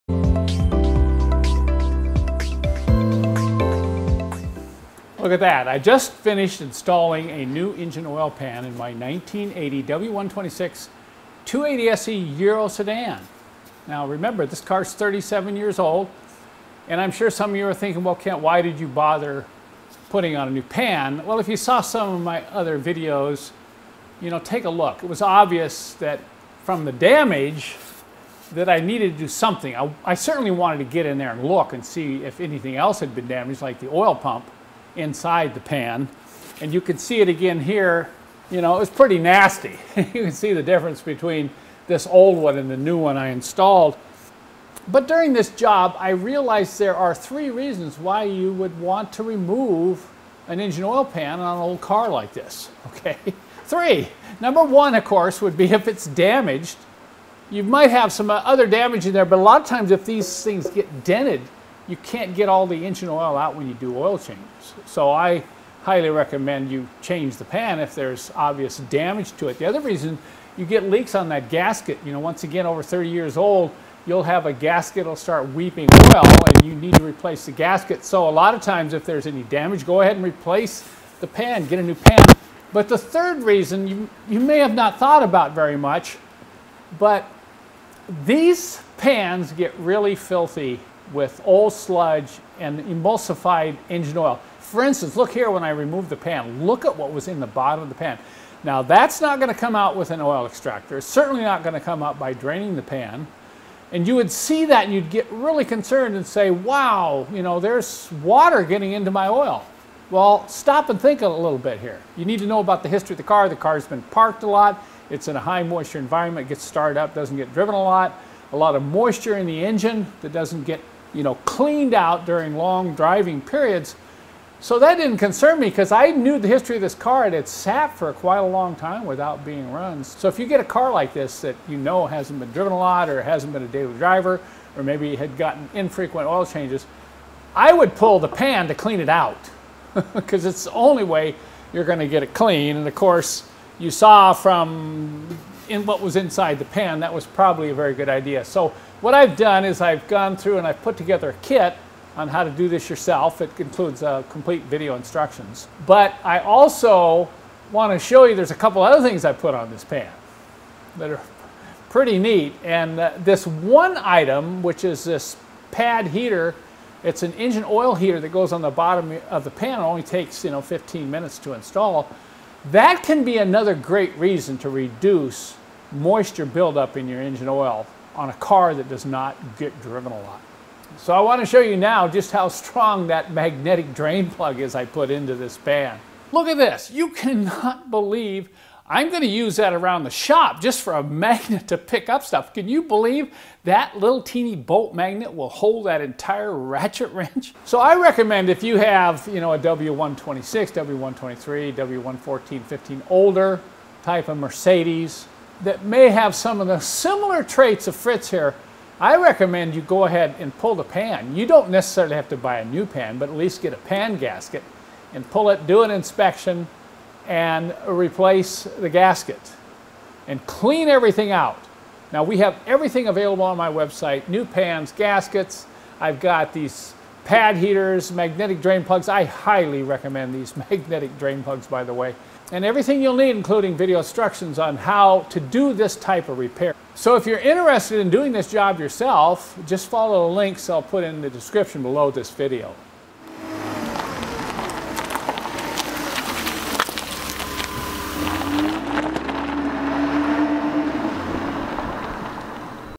Why You Should Remove the Oil Pan on an Old Car
There are at least 3 good reasons that I will share with you as I remove an oil pan from a 1980 W126 280SE sedan. Link to replacement parts